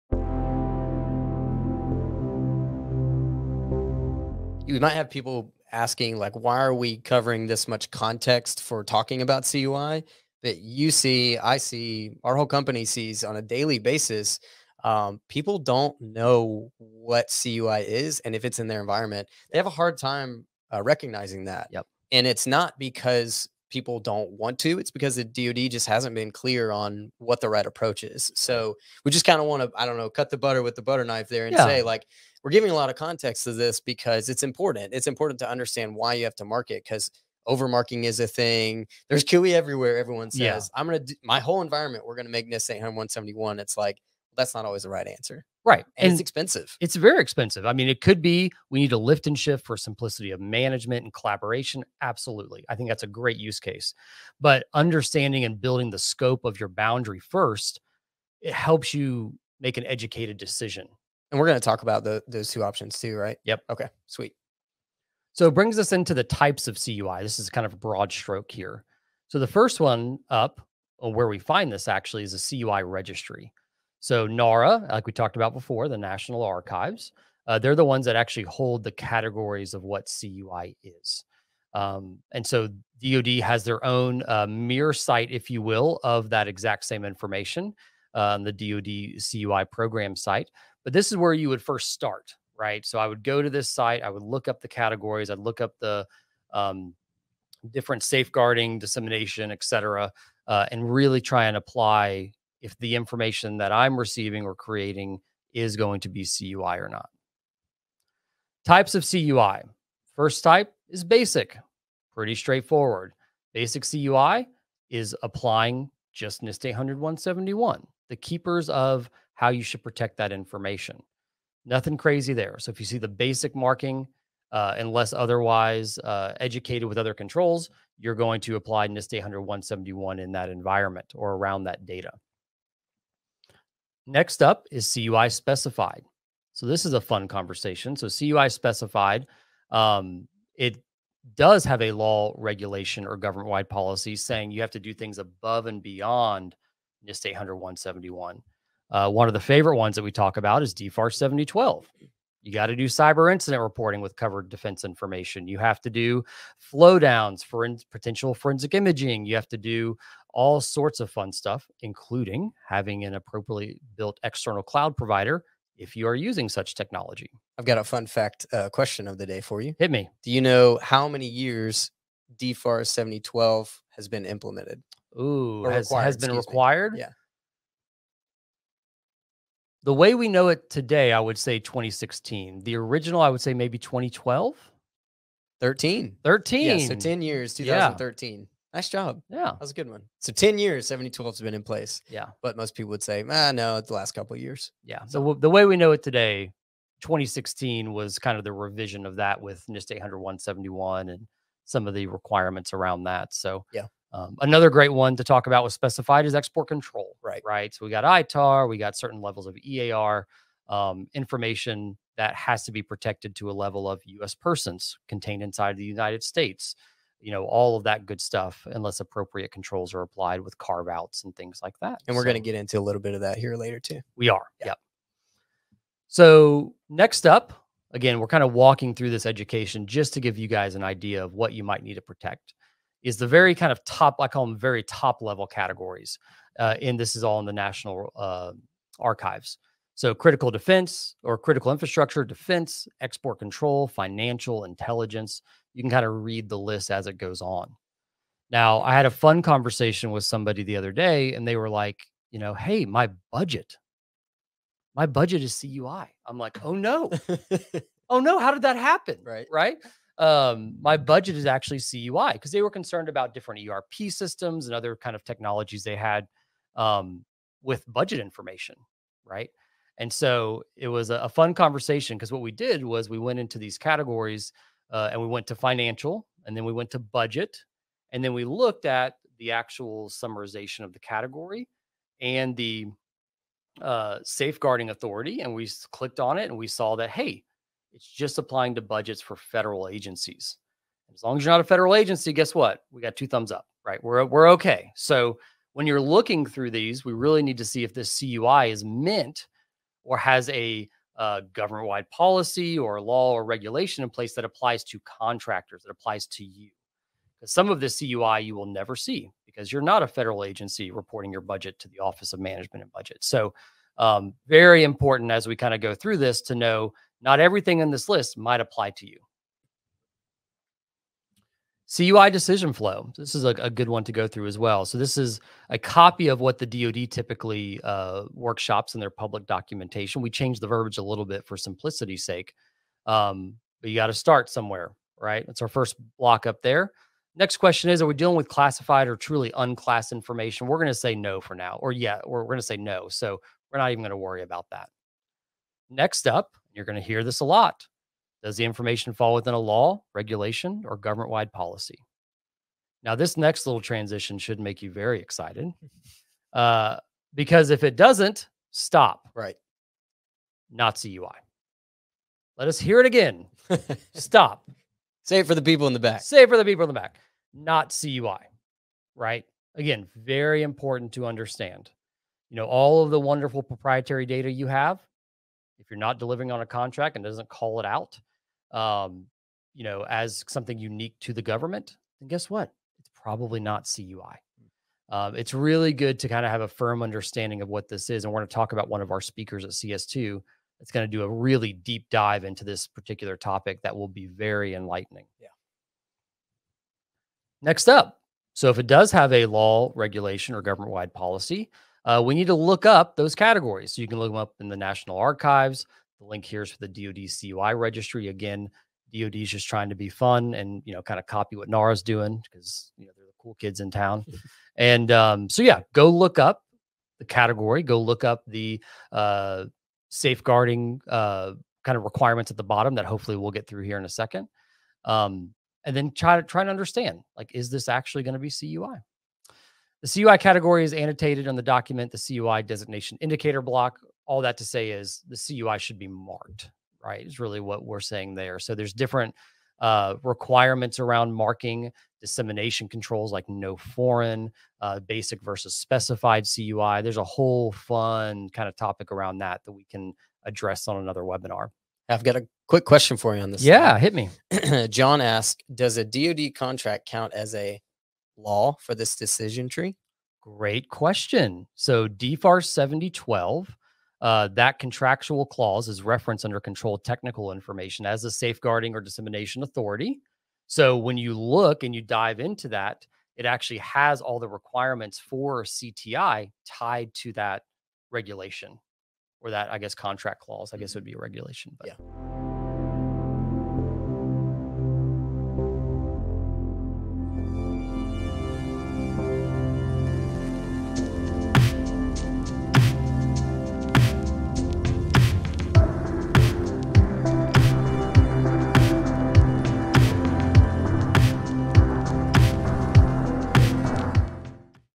Types Of CUI
In this clip from the recent Summit 7 webinar, "Protecting CUI with Microsoft 365", Sam & Daniel talk about why cover this much context about CUI, the different types, categories and decision flow.

Don't want to miss out on our next webinar?

Want to learn more about CUI?

Video Chapters:
(0:00-1:05) - Why cover this?
(1:06-3:47) - Types of CUI
(3:48-5:20) - CUI categories
(5:21-8:01) - CUI decision flow